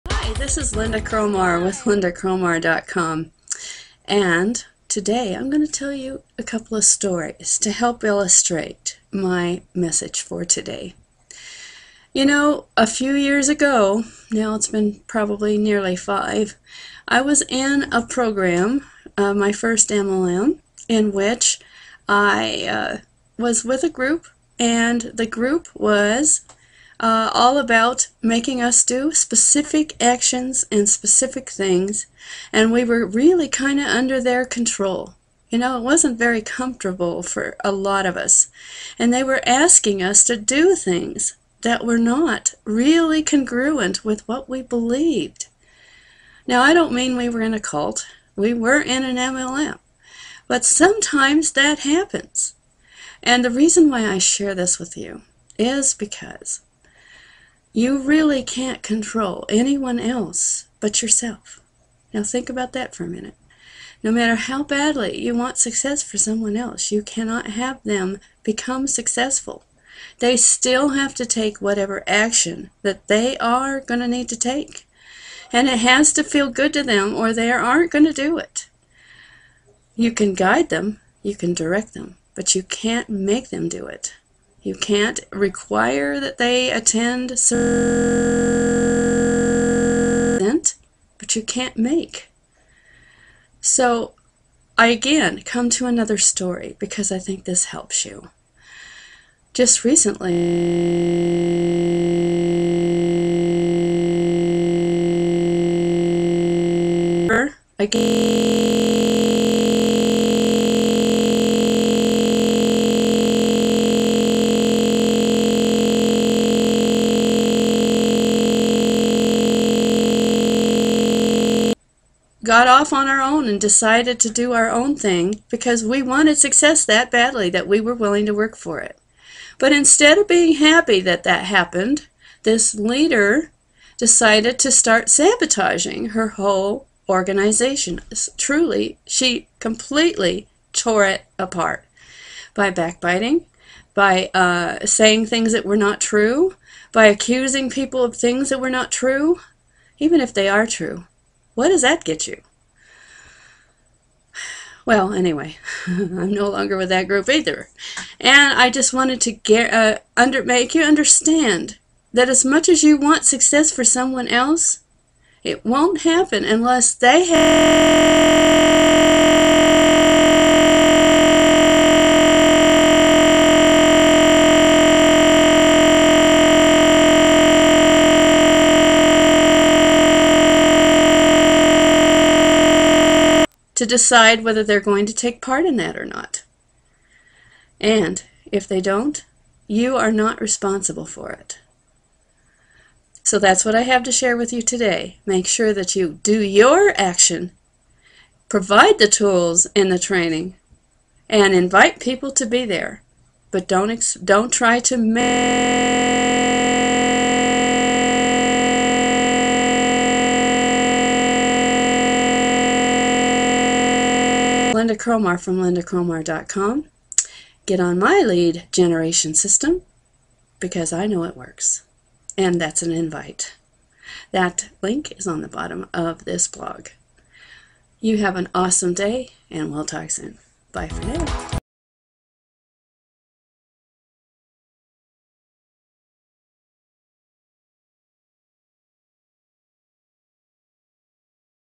You Can Invite and Provide Training and Coaching Not Prodding!
You Can Invite and Provide Training and Coaching Not Prodding was originally published online in  2011 on another channel.. I am bringing it back here to preserve it..